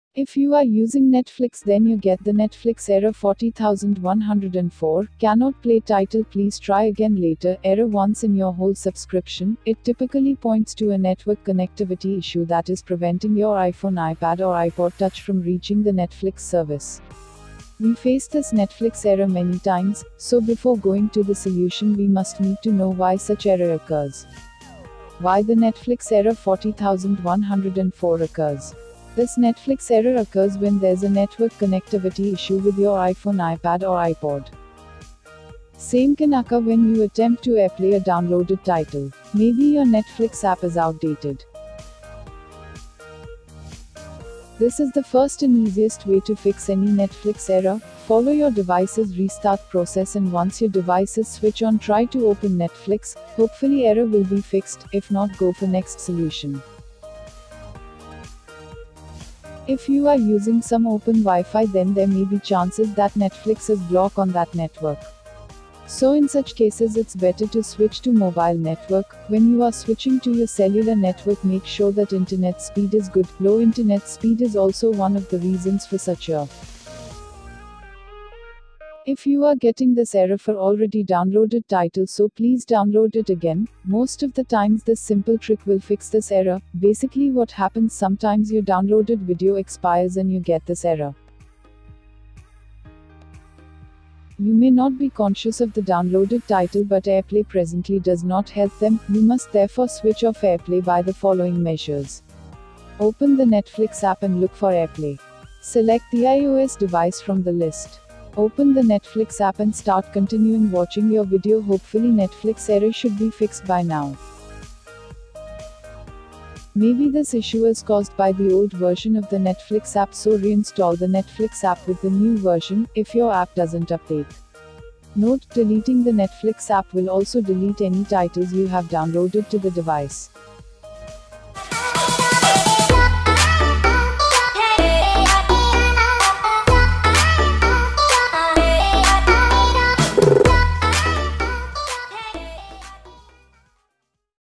How to fix Netflix Error 40104- ‘Cannot play title. Please try again later. (40104)’
These are some methods to solve the Netflix Error 40104 ‘Cannot play title. Please try again later. (40104)’. Please let us know if you have closely followed all the methods and are still getting the same wrong. You will also be able to comment on your queries below.
Solution 1: Restart Your iPhone, iPad or iPod
Solution 2: Check your Network Connectivity
Solution 3: Download the Title Again
Solution 4: Turn Off Airplay
Solution 5: Reinstall the Netflix App